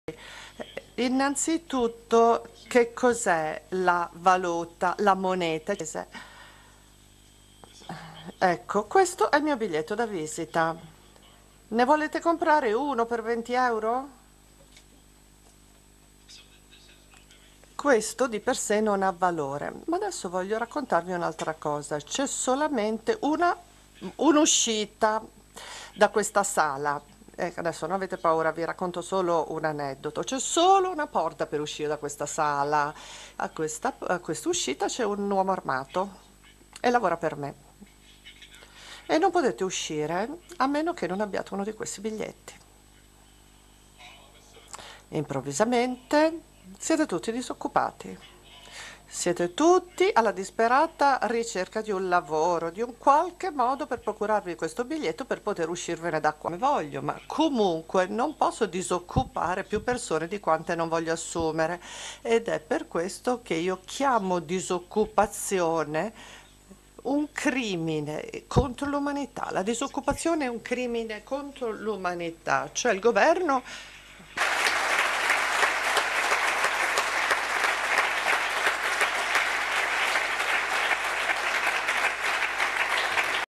Warren Mosler: Che cos'è la moneta, e la disoccupazione
Martedì 18 Giugno 2013 alle ore 21, si incontrano all'auditorium confesercenti di Siena, L'economista Warren Mosler, insieme al giornalista Paolo Barnard, in occasione del tour: Salvare l'Italia.
L'incontro tratta della crisi della zona euro, e delle soluzioni strutturali per il rilancio dell'economia, e la piena occupazione.

Nel filmato vediamo alcuni frammenti dell'incontro tenutosi in Sardegna nell'ottobre del 2012: Torneremo Italia.